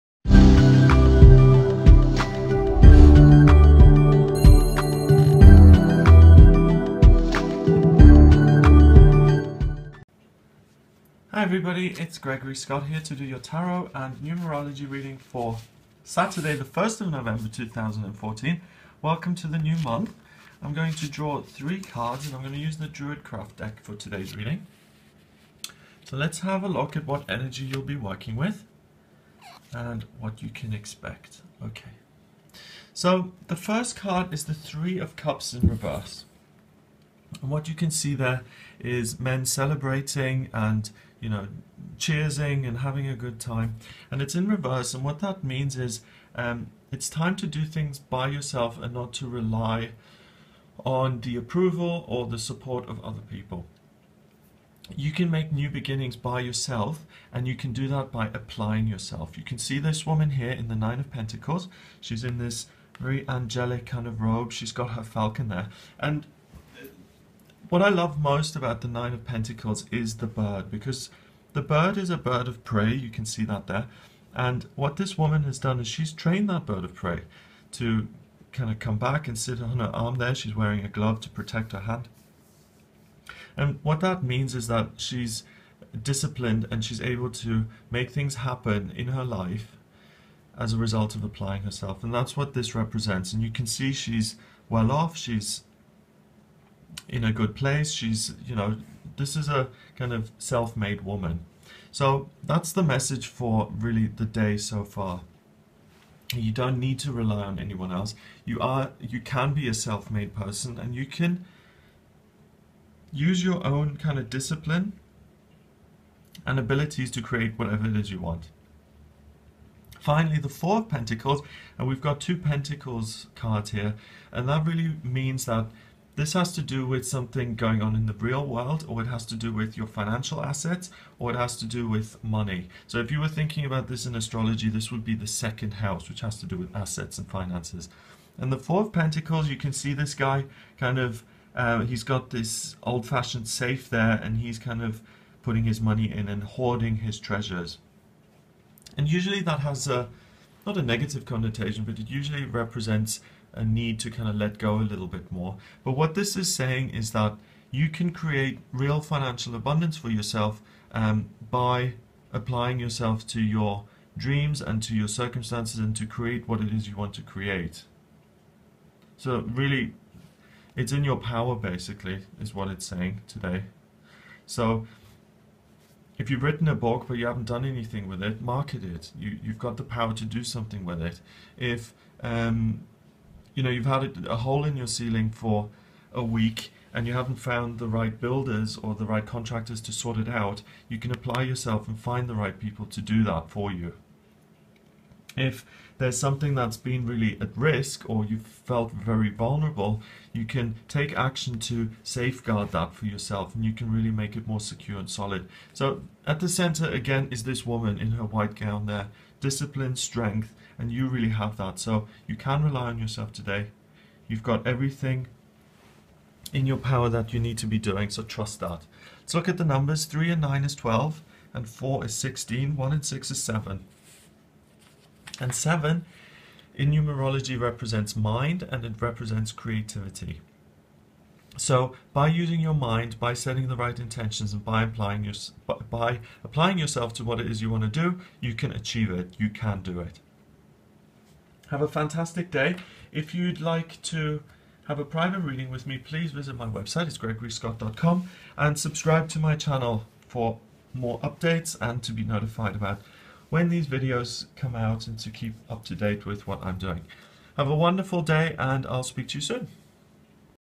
Tarot Numerology Reading 1 November 2014
↓ Open the description ↓

What energy will you be working with today? Find out in this free tarot and numerology reading for 1 November 2014.

Tarot Card and Numerology Meanings:

It’s a great time to do things by yourself without the need for approval from other people.

Use self-discipline and realise that it’s safe to rely on your own skills and abilities. It’s in your power to create abundance in your life and to improve your financial circumstances.

Today’s numerology vibration is: 7 = your mind is the co-creator of your reality. Be imaginative!